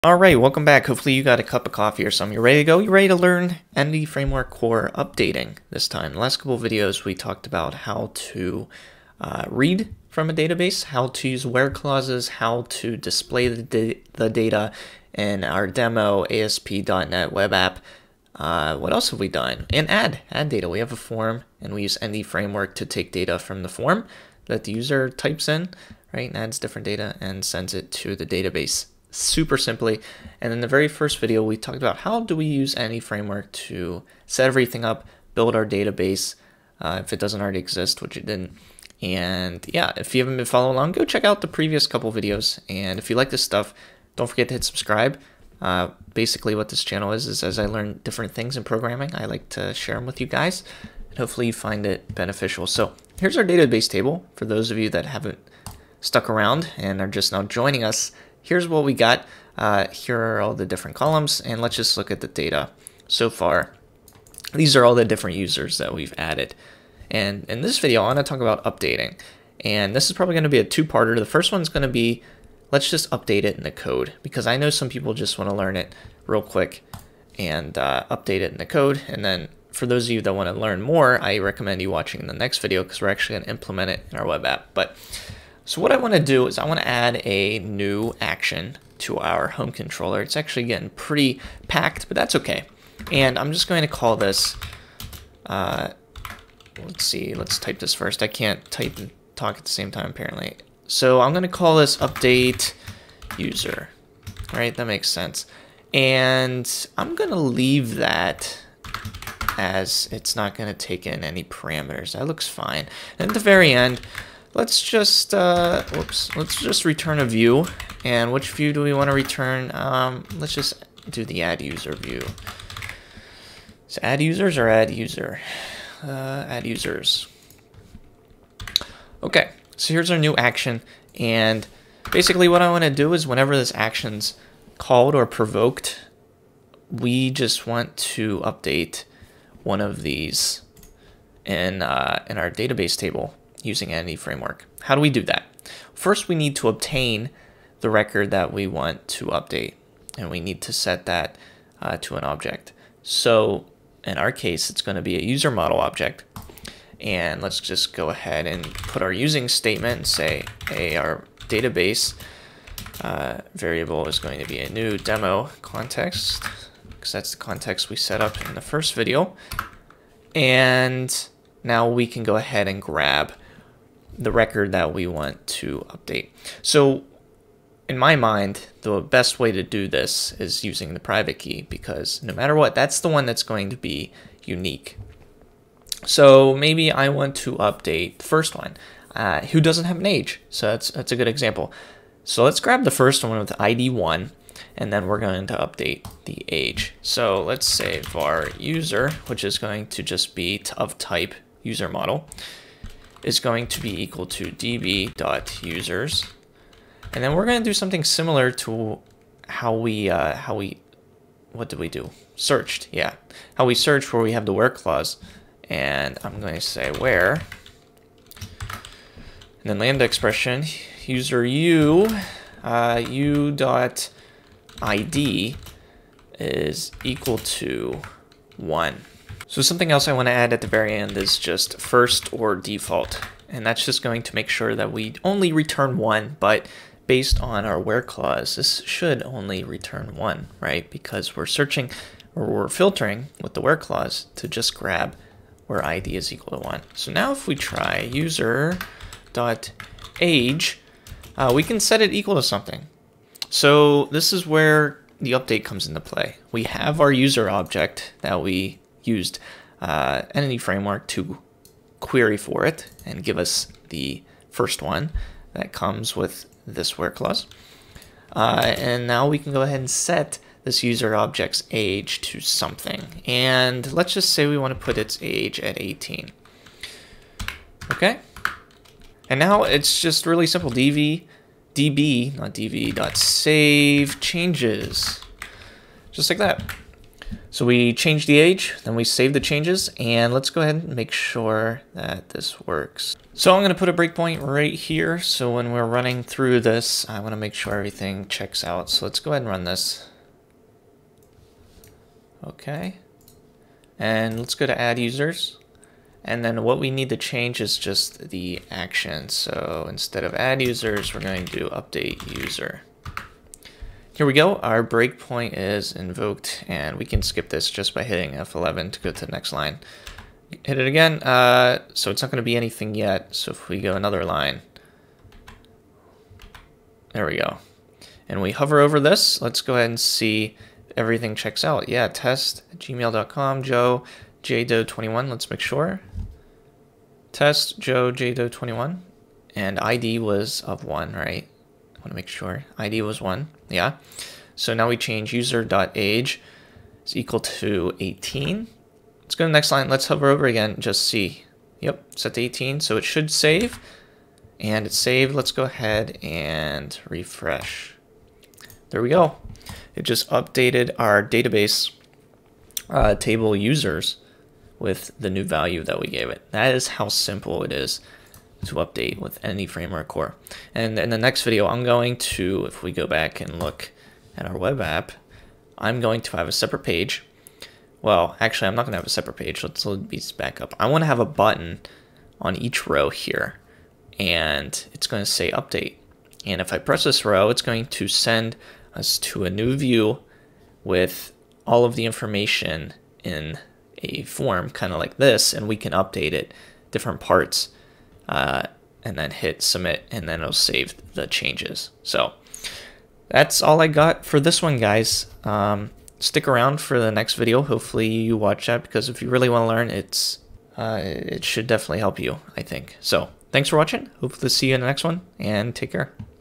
How to Update a Row in your Database with Entity Framework Core - Entity Framework Core Tutorial
The Standing Desk I Use